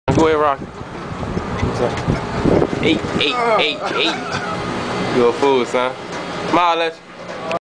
My cuzzo Rouse on his gogo shit walking thru a p-way on the ike! lls
My cuzzo Rouse was always on his gogo shit on and off the ship. 2live! production